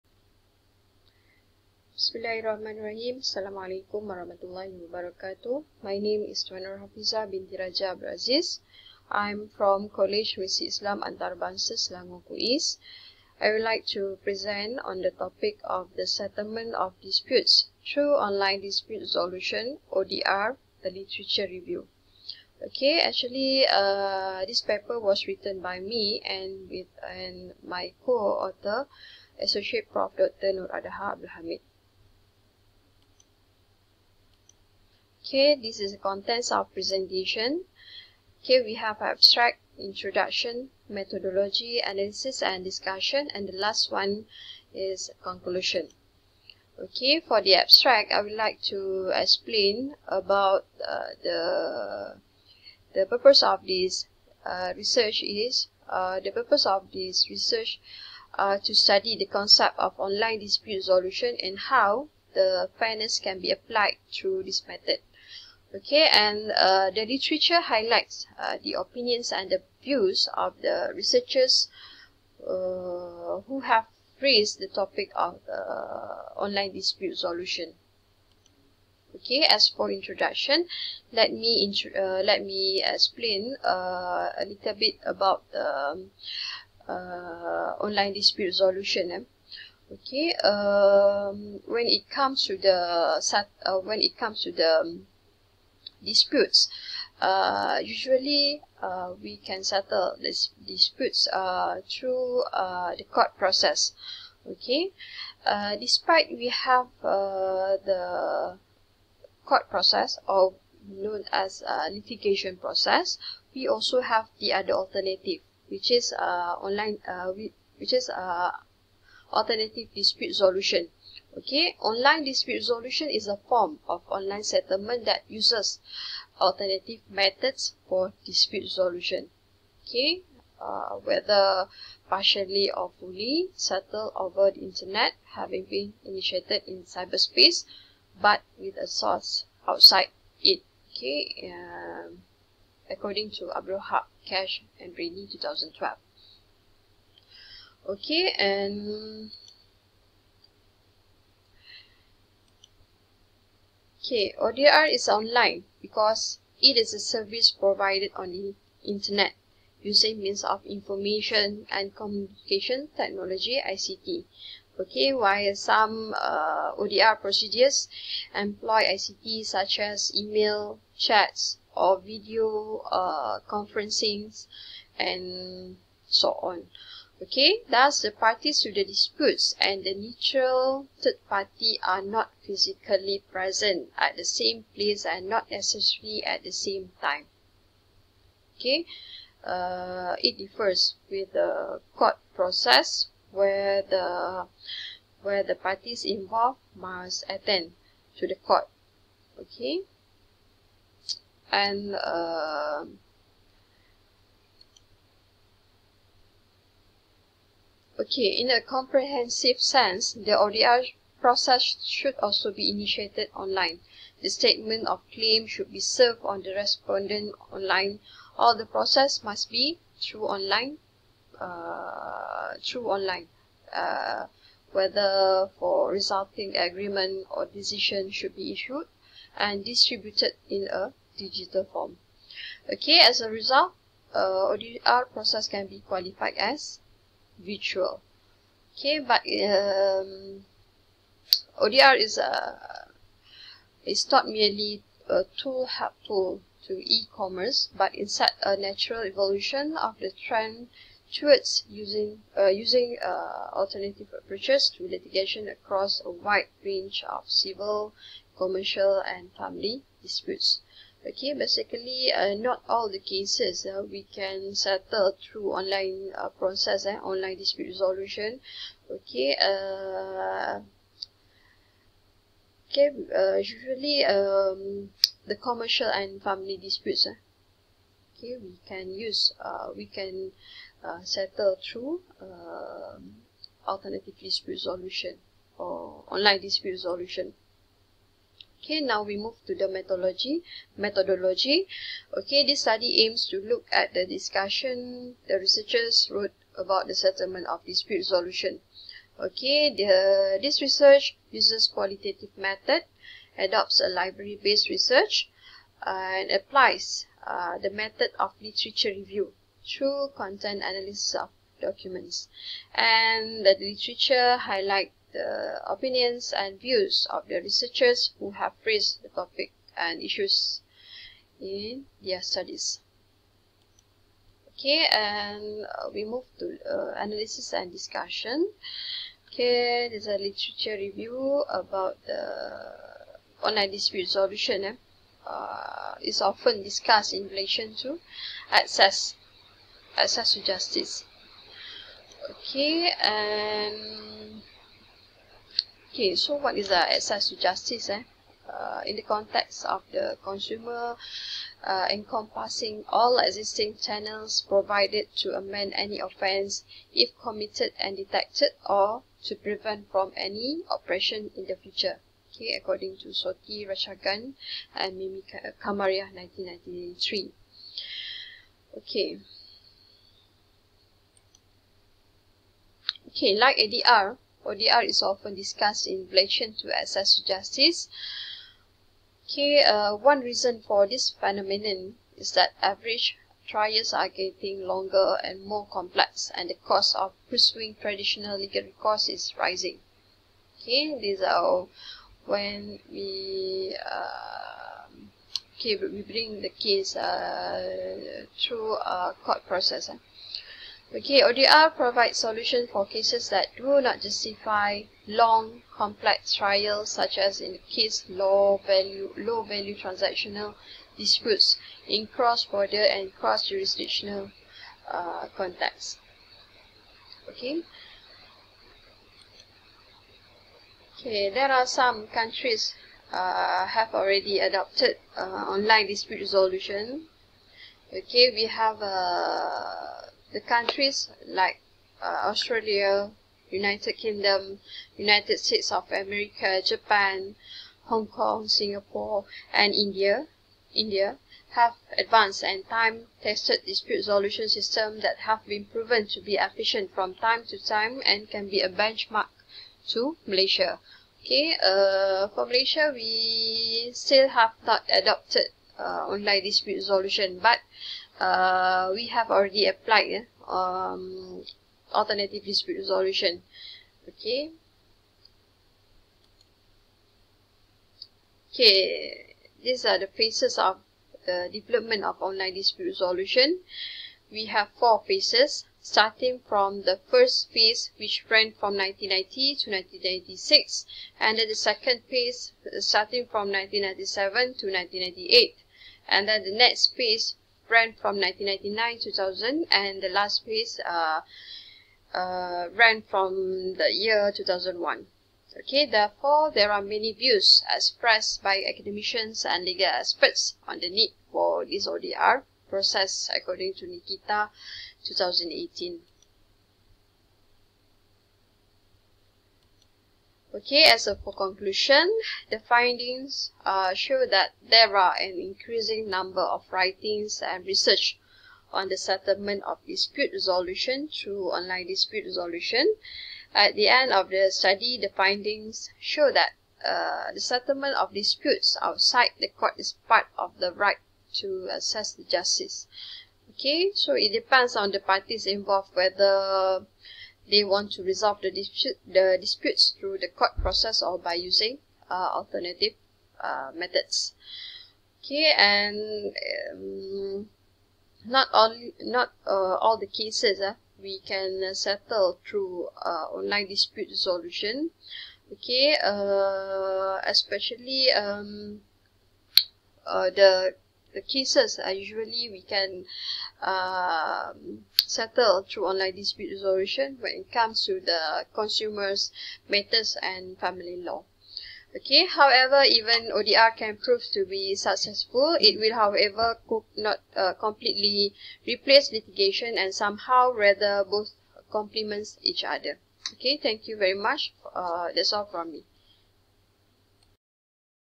The Settlement of disputes through Online Dispute Resolution ODR: A Literature Review
Presenter: Tuan Nurhafiza Raja Abdul Aziz
Institution: Kolej Universiti Islam Antarabangsa Selangor